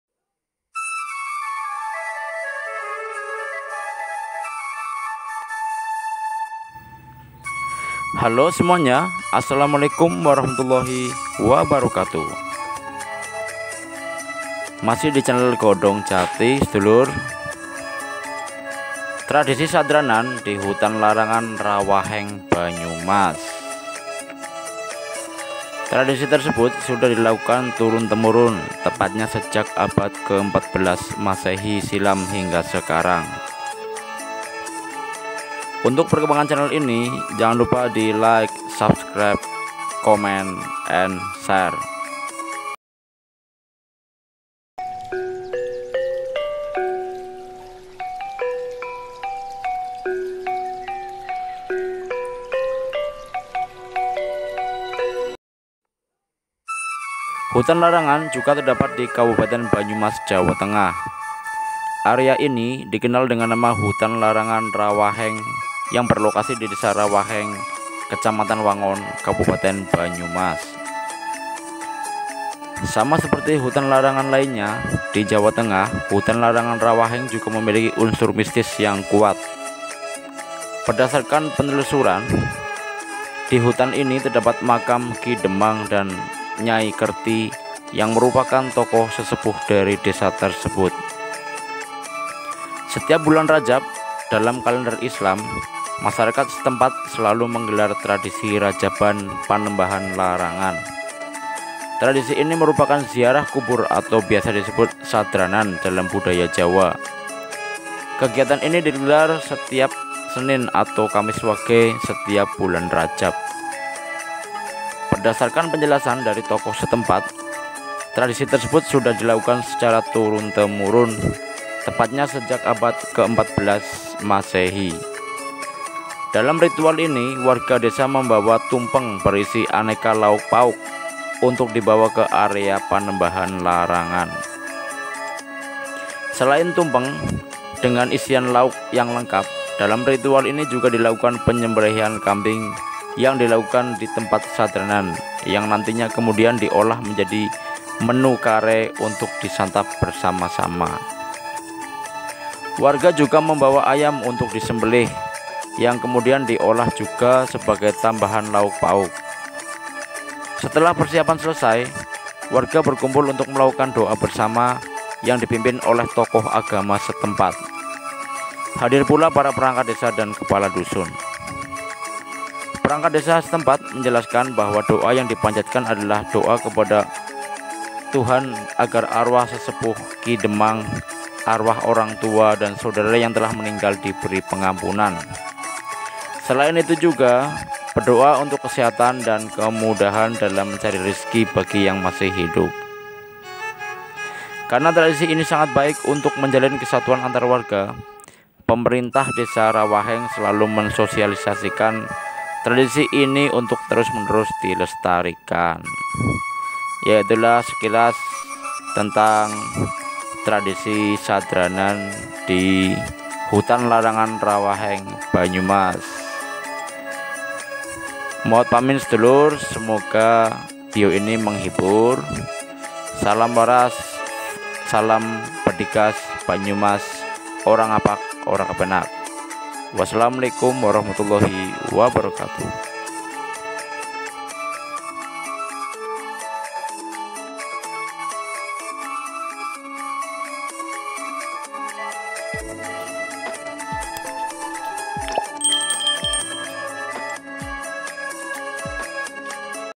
Tradisi Menjelang Ramadhan "SADRANAN", Tradisi Nenek Moyang Banyumasan sejak 14 Masehi
BANYUMAS — Hutan Larangan juga terdapat di Kabupaten Banyumas, Jawa Tengah. Area ini dikenal dengan nama Hutan Larangan Rawaheng yang berlokasi di Desa Rawaheng, Kecamatan Wangon, Kabupaten Banyumas.

Sama seperti hutan larangan lainnya di Jawa Tengah, Hutan Larangan Rawaheng juga memiliki unsur mistis yang kuat. Berdasarkan penelusuran Senin (21/3/2022), di hutan ini terdapat makam Ki Demang dan Nyai Kerti yang merupakan tokoh sesepuh dari desa tersebut.

Setip bulan Rajab dalam kalender Islam, masyarakat setempat selalu menggelar tradisi Rajaban Panembahan Larangan. Tradisi ini merupakan ziarah kubur atau biasa disebut sadranan dalam budaya Jawa. Kegiatan ini digelar setiap Senin atau Kamis Wage setiap bulan Rajab.

Berdasarkan penjelasan dari tokoh setempat, tradisi tersebut sudah dilakukan secara turun-temurun, tepatnya sejak abad ke-14 Masehi. Dalam ritual ini, warga desa membawa tumpeng berisi aneka lauk pauk untuk dibawa ke area Panembahan Larangan.

Selain tumpeng dengan isian lauk yang lengkap, dalam ritual ini juga dilakukan penyembelihan kambing yang dilakukan di tempat sadranan yang nantinya kemudian diolah menjadi menu kare untuk disantap bersama-sama.

Warga juga membawa ayam untuk disembelih yang kemudian diolah juga sebagai tambahan lauk pauk. Setelah persiapan selesai, warga berkumpul untuk melakukan doa bersama yang dipimpin oleh tokoh agama setempat. Hadir pula para perangkat desa dan kepala dusun.

Perangkat desa setempat menjelaskan bahwa doa yang dipanjatkan adalah doa kepada Tuhan agar arwah sesepuh Ki Demang, arwah orang tua dan saudara yang telah meninggal diberi pengampunan. Selain itu juga berdoa untuk kesehatan dan kemudahan dalam mencari rezeki bagi yang masih hidup.

Karena tradisi ini sangat baik untuk menjalin kesatuan antar warga, Pemerintah Desa Rawaheng selalu mensosialisasikan tradisi ini untuk terus dilestarian keberadaannya.